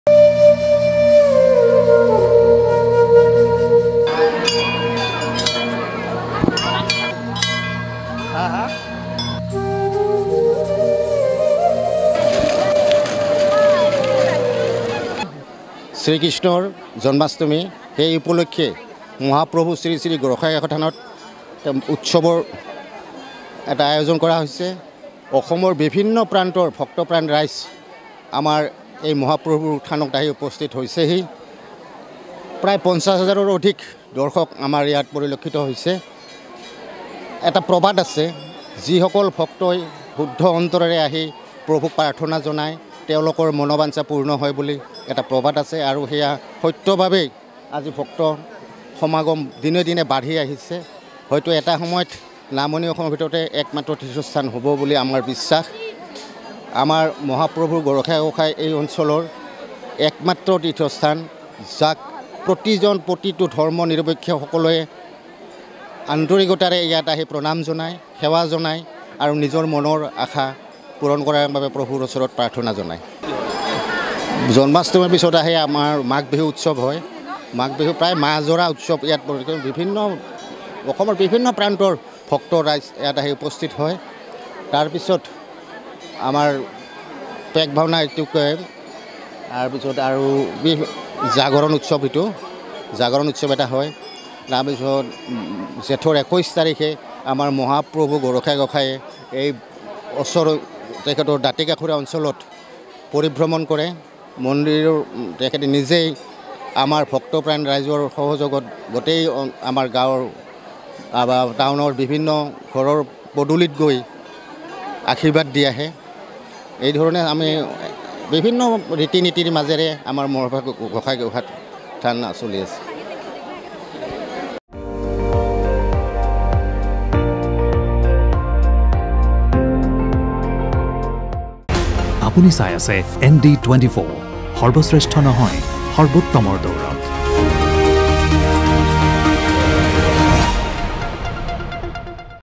Barpeta News: বৰপেটাৰ সৰভোগৰ ঐতিহাসিক শ্ৰীশ্ৰী গৰখীয়া গোসাঁই থানত শ্ৰীকৃষ্ণৰ জন্মাষ্টমী পালন
বৰপেটাৰ সৰভোগৰ ঐতিহাসিক শ্ৰীশ্ৰী গৰখীয়া গোসাঁই থানত শ্ৰীকৃষ্ণৰ জন্মাষ্টমী পালন।  গোসাঁই থান চৌহদত ভিৰ অগণন ভক্তৰ।